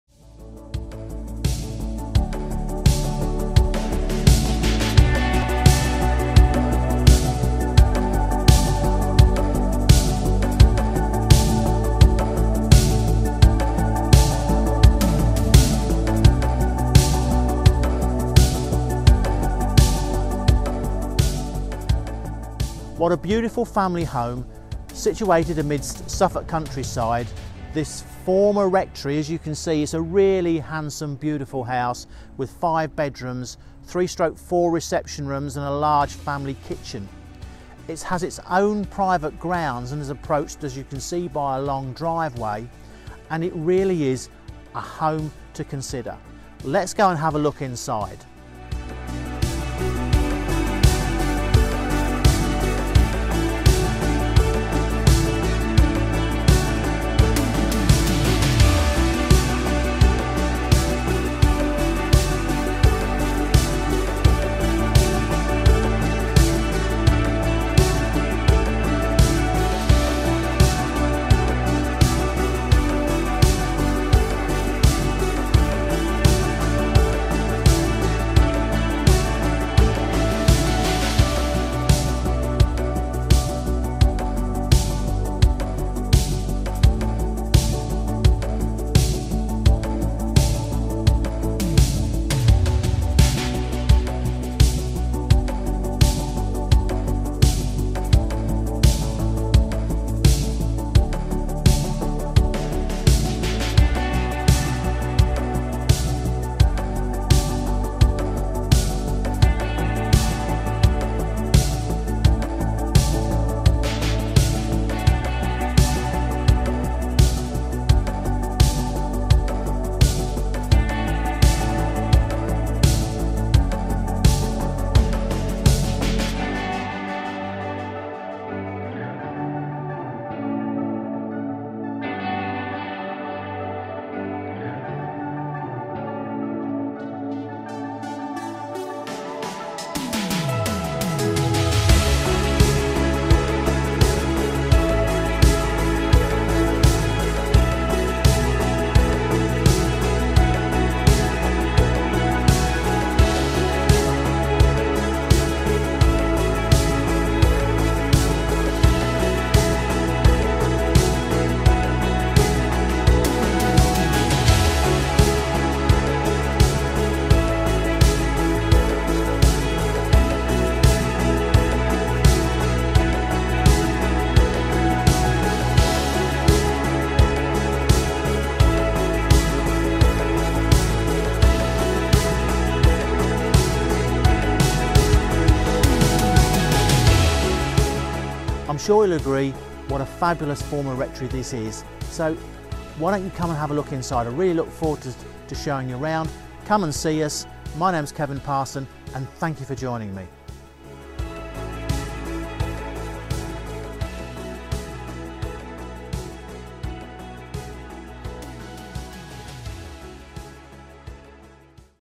For Sale in Denham - Superb Former Rectory in Grounds of 1.4 acres
Handsome Detached House, with 4 Reception Rooms, 5 Bedrooms and 2 Bathrooms. Fabulous property, a must view.